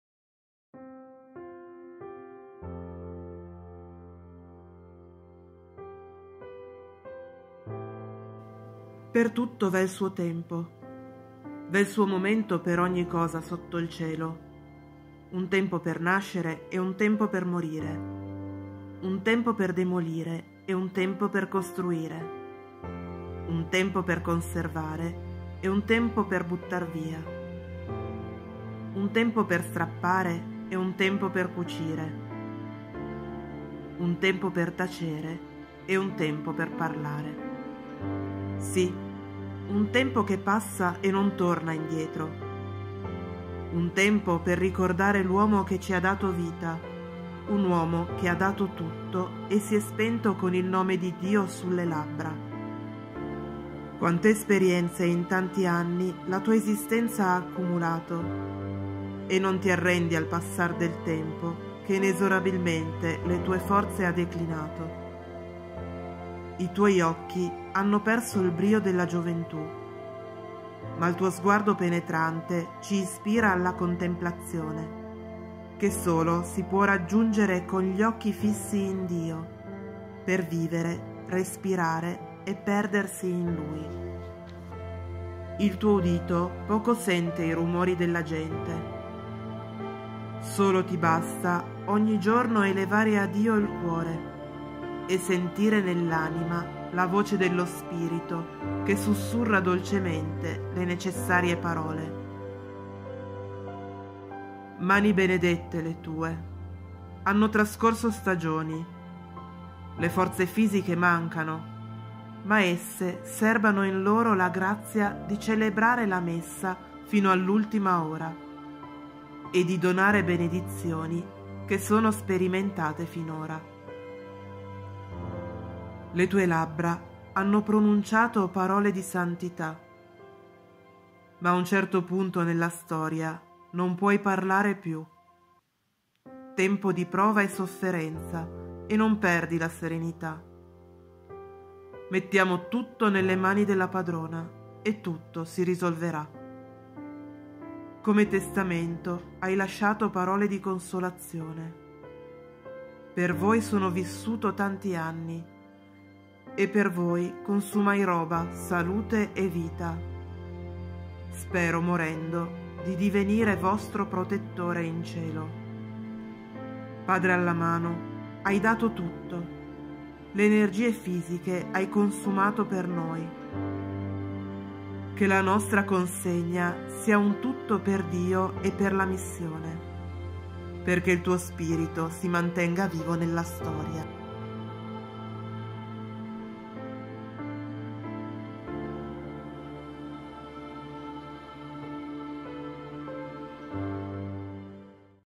Giuseppe Allamano, ci hai dato tutto
Un tempo per ricordare l’uomo che ci ha dato vita,
un uomo che ha dato tutto,
e si è spento con il nome di Dio sulle labbra.
BUONA FESTA DEL BEATO GIUSEPPE ALLAMANO